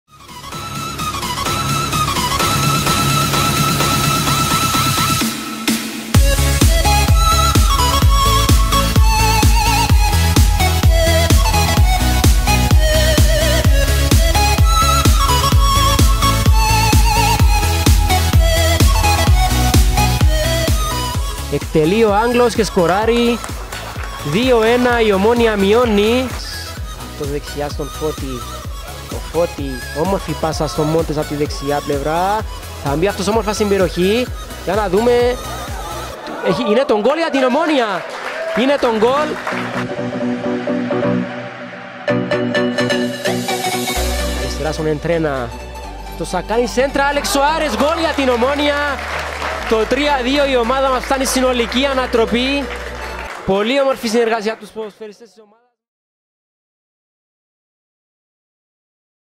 Τα γκολ της ΟΜΟΝΟΙΑΣ στο φιλικό με τον Ερμή
Παρακολουθήστε τα γκολ των Νταρμπισάιαρ (πεν.), Μόντες και Άλεξ, στο φιλικό παιχνίδι με τον Ερμή